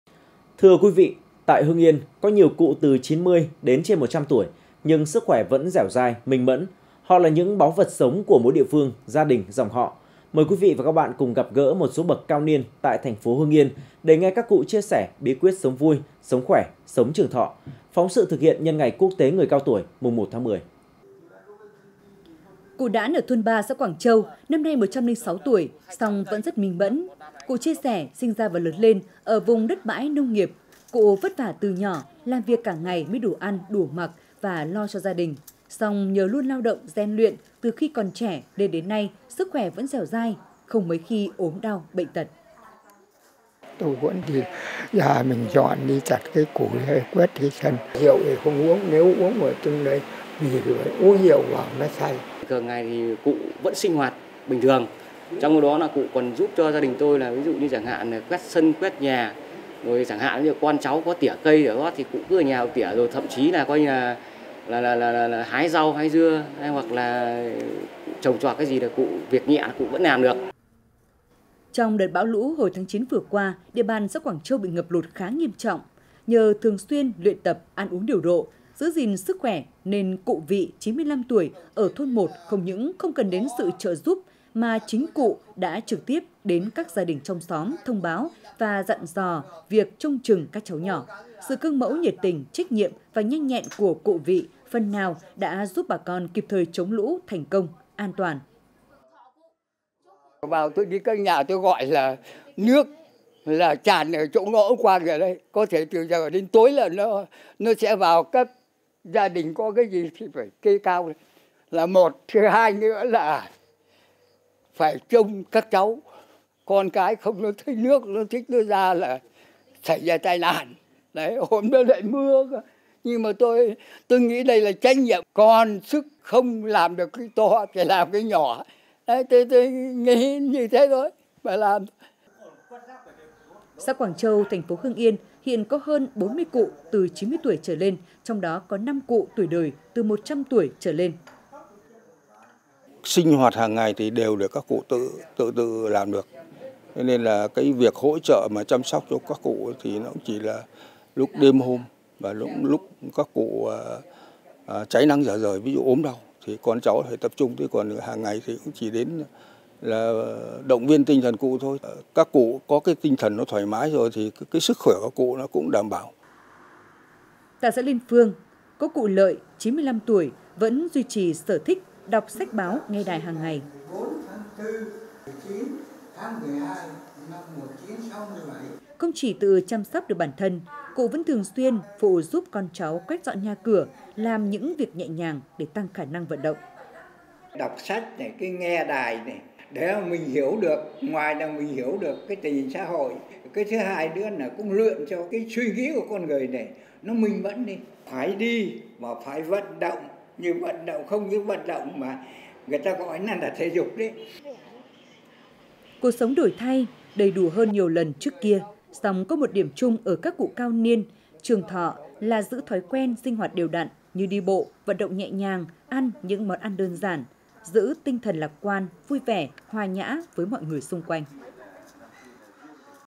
Bí quyết sống trường thọ của các bậc cao niên ở thành phố Hưng Yên
Hưng Yên TV | Bí quyết sống trường thọ của các bậc cao niên ở thành phố Hưng Yên

Tiktok: Truyền hình Hưng Yên
Facebook: Đài Phát thanh và Truyền hình Hưng Yên
Zalo: Đài PTTH Hưng Yên